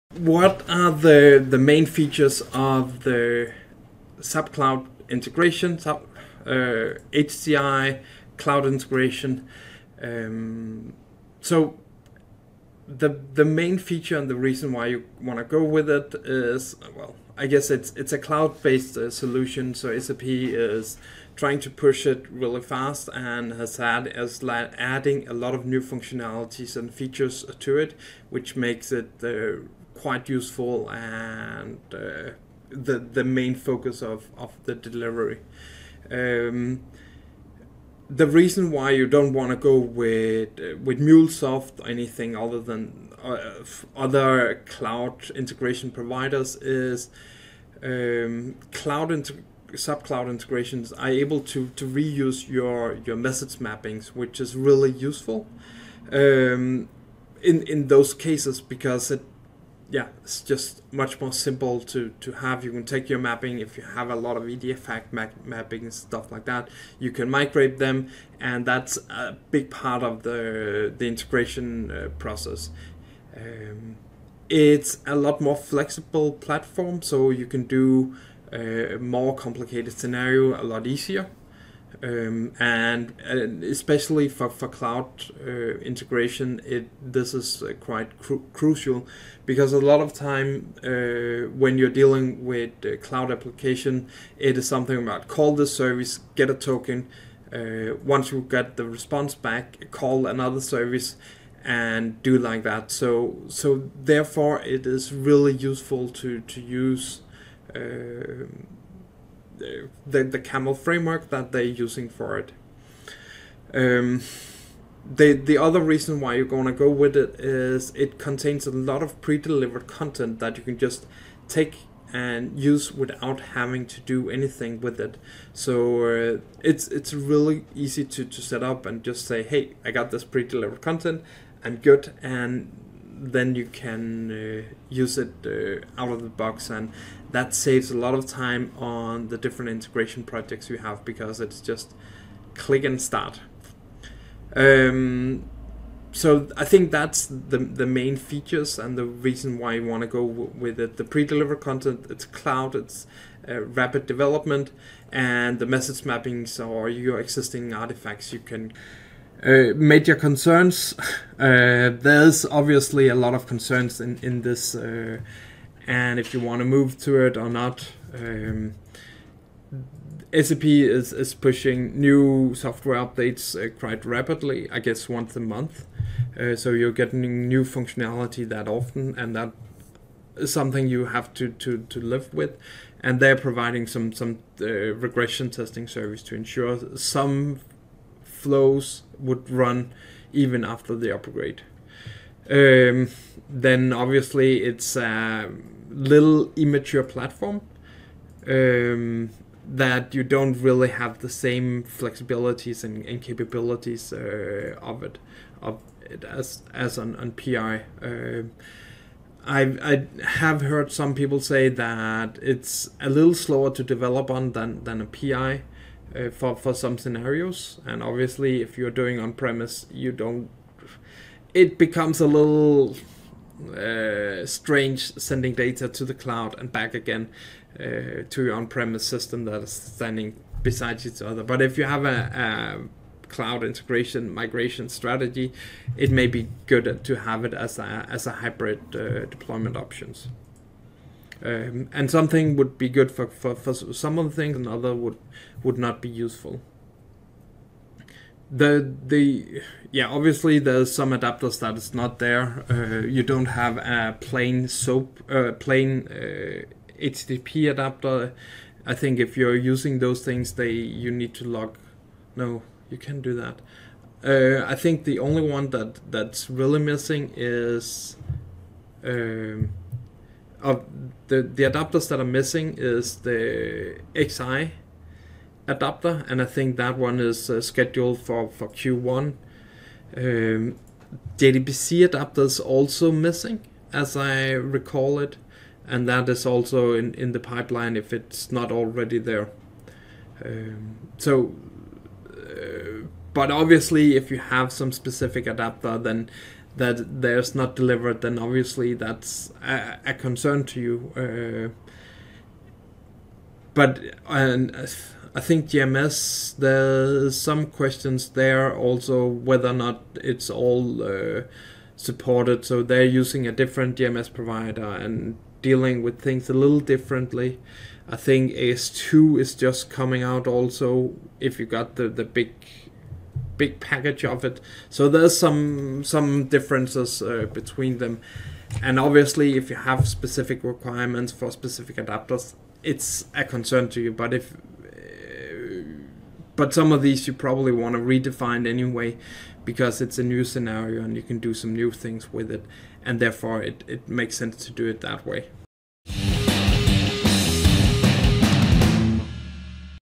What are the main features of SAP Cloud Integration HCI/CPI
The main reasons why you want to go with SAP Cloud Platform Integration(CPI) is the following.
It is a cloud-based solution so there is rapid development going on
It is able to leverage your investments in Message Mapping so you can reuse the same code for it.
It is a more flexible platform for integration. You don’t need to create BPM processes for handling most Cloud integrations that require getting tokens and updating in multiple HTTP/SOAP Rest Calls
It has a lot of pre-delivered content. So you can get started with the integration a lot faster.

If you are looking training in SAP PI/PO check out my courses
If you have any SAP Integration questions join and learn when next is.